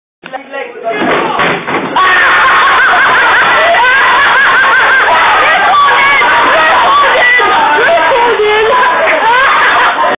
The pigeon that cant fly - liam pagones
Form 1 morning liam thought he was being clever and tried to jump over some neek... ollie pushed the table into his legs... heres the result :)